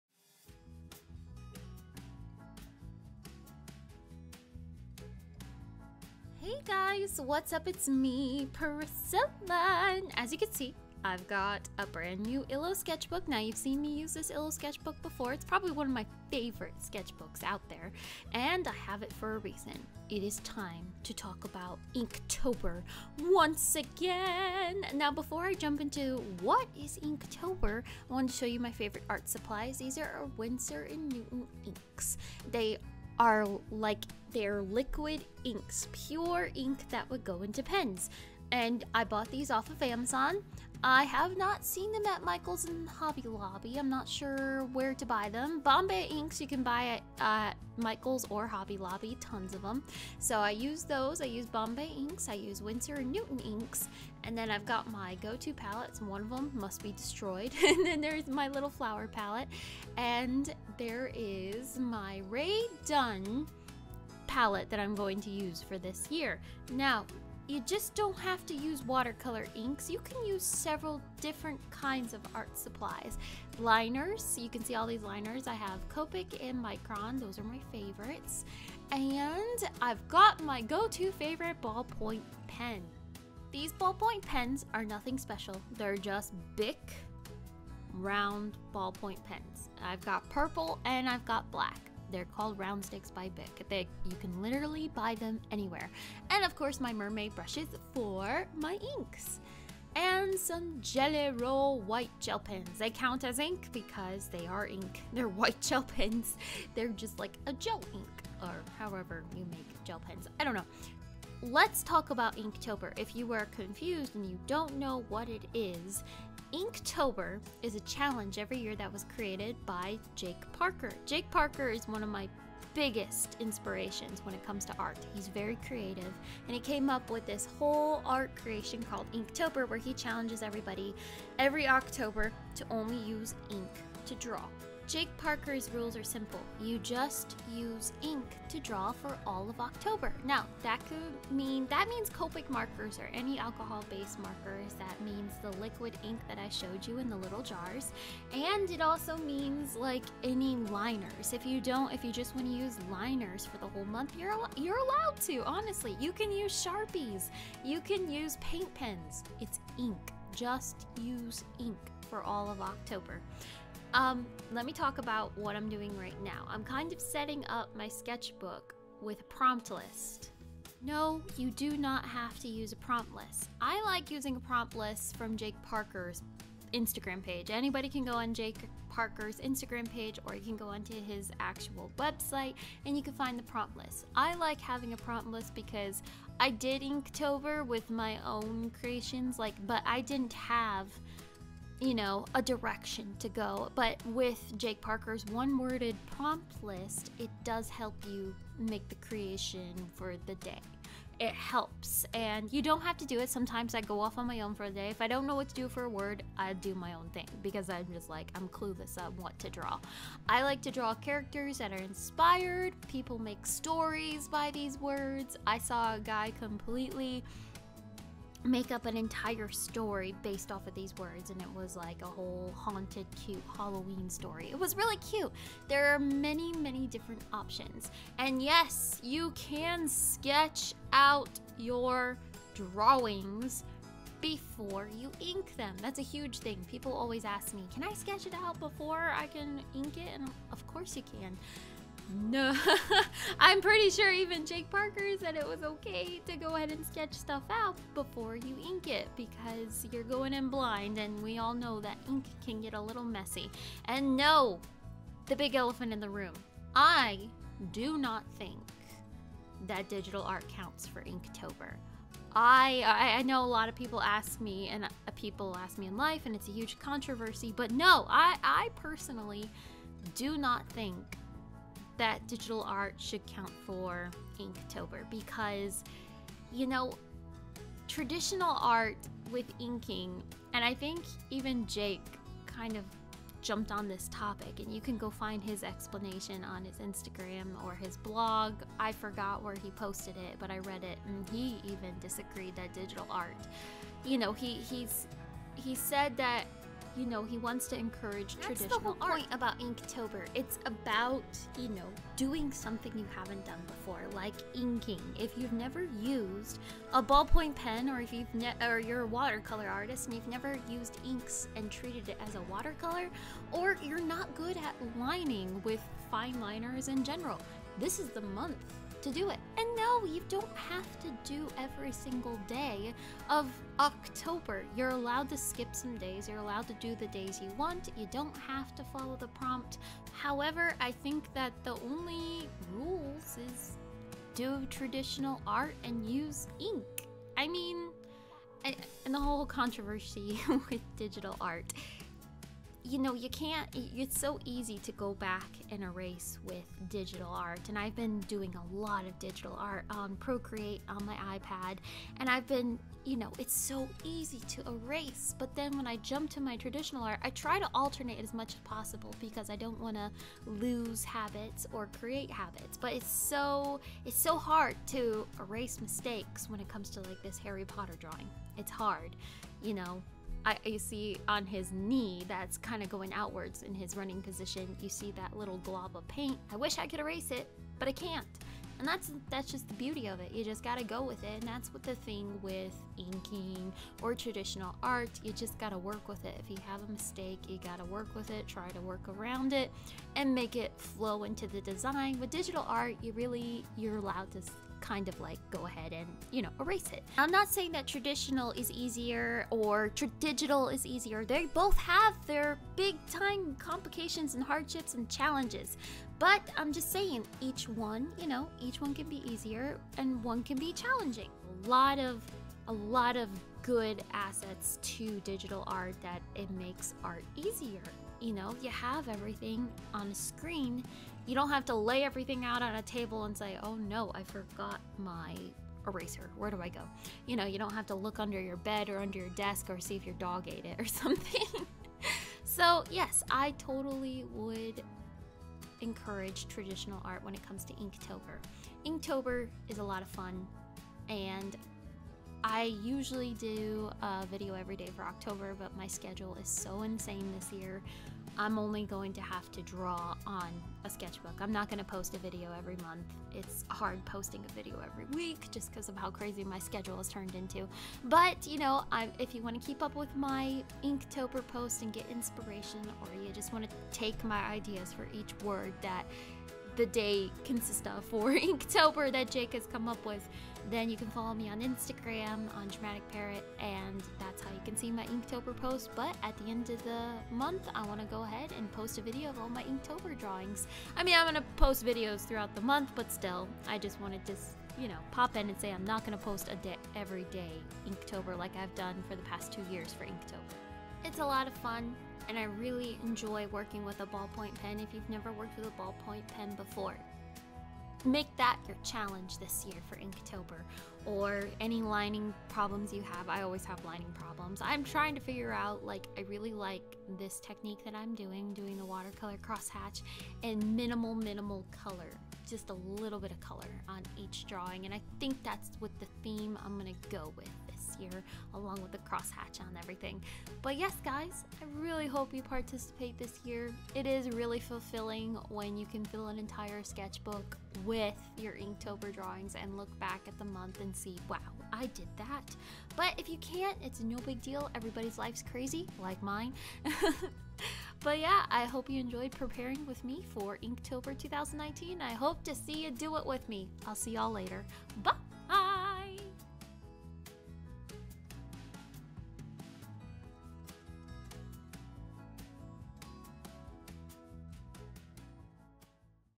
PREPARE WITH ME for INKTOBER 2019
Here are my supplies and plans for Inktober 2019! We are half way through September and October is just around the corner. That means INKTOBER 2019!

If you have never been a part of Inktober I explain a little about Inktober in this video and exactly what supplies I plan to use. What supplies you can use for Inktober!

I also answer the question, "is digital art acceptable for Inktober?" Well. I answer that in this video, my personal opinion.